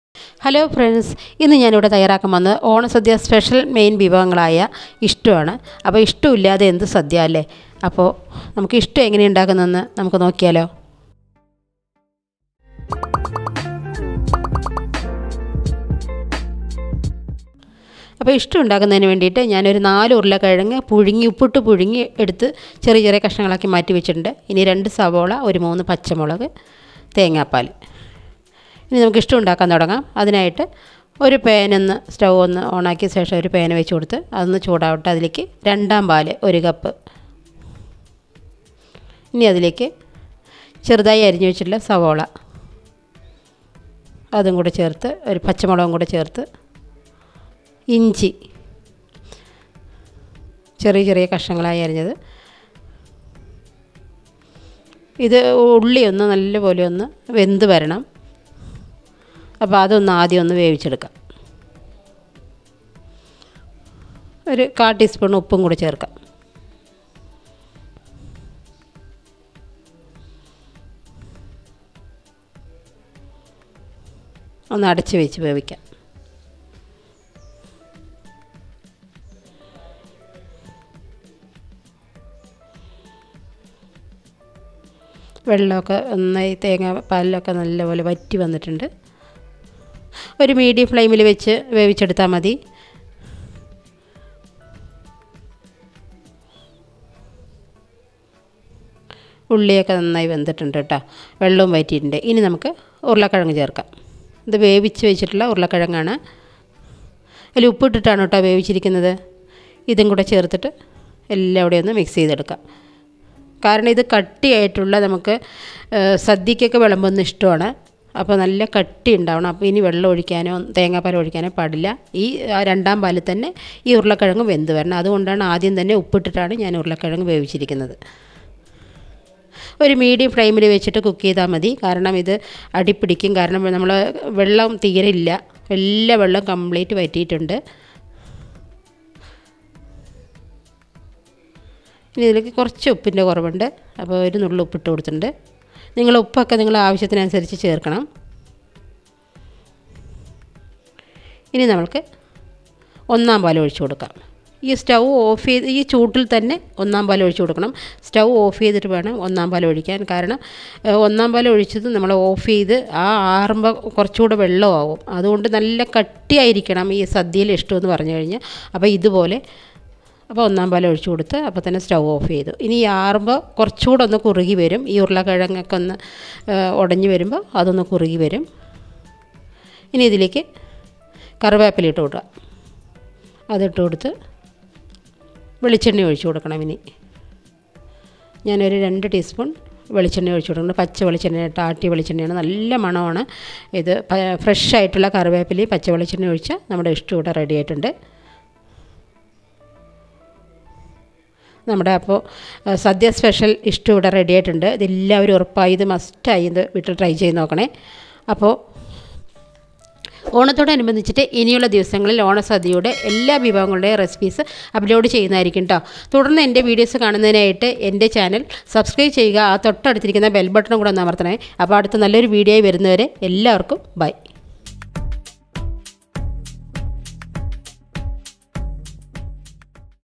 ഉരുളകിഴങ്ങു ഇഷ്ട്ടു ഓണം സദ്യ SPECIAL | Malayalam Recipe
Potato stew recipe with step by step photos. This is a Kerala style potato stew or Ishtu is made with coconut milk, pearl onions, herbs and spices. It makes for a comforting and delicious meal when paired with appam or idiyappam or ghee rice. A vegan and gluten free ❣️